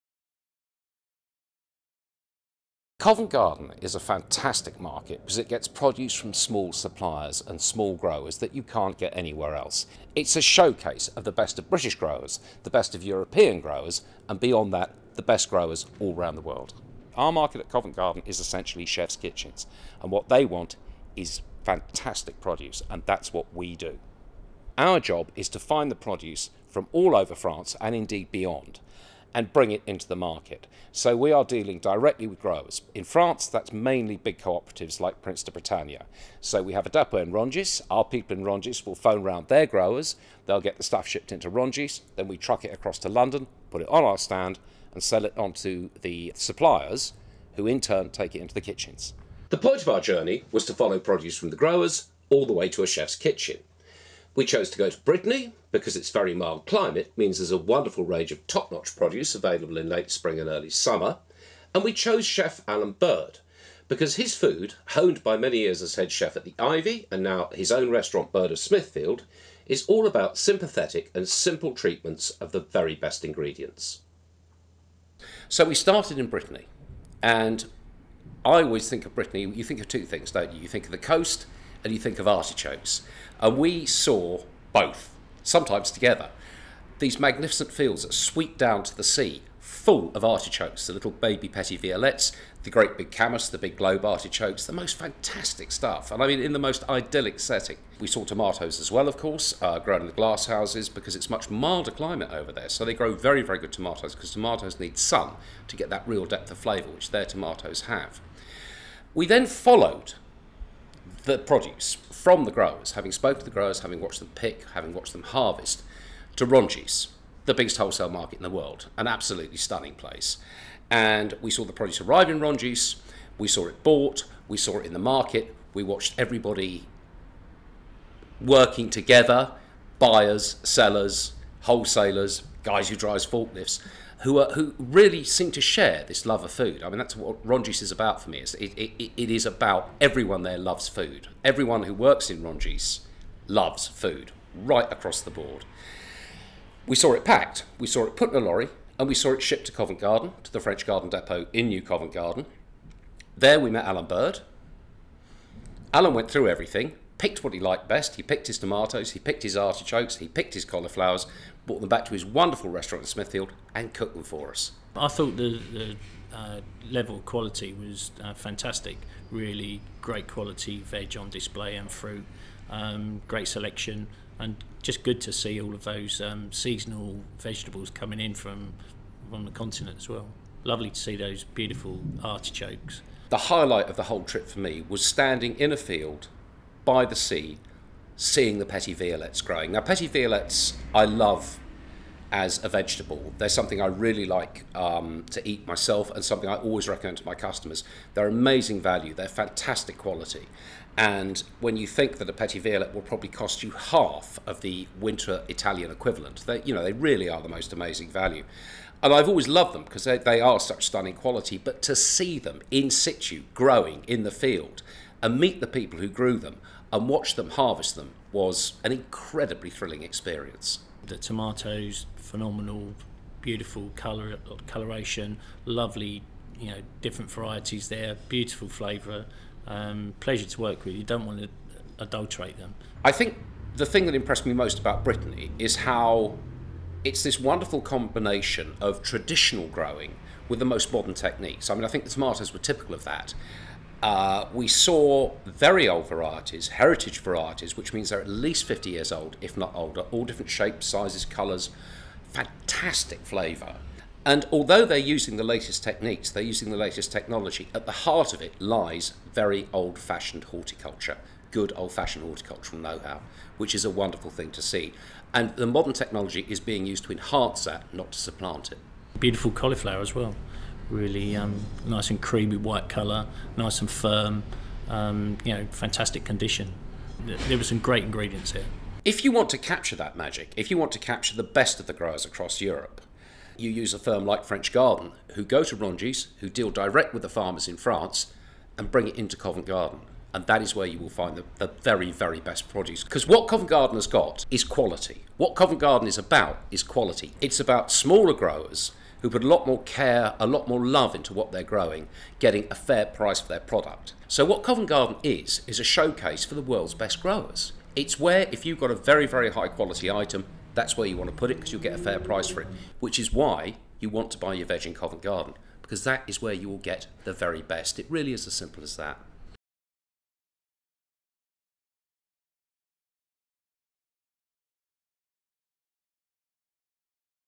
Bringing France to New Covent Garden Market with The French Garden
We teamed up with New Covent Garden Market wholesaler The French Garden to visit Brittany, France, to film the journey of produce from the grower all the way to the chef's kitchen here in London.

In our video we follow the story of artichokes, cauliflowers and tomatoes grown on the French coastline, which are transported to Rungis wholesale market in Paris and then on to The French Garden here at the Market.

Back at the Market we join chef Alan Bird, Bird of Smithfield, who hand picks the finest ingredients to take back to his restaurant, where he transforms the produce into three stunning dishes.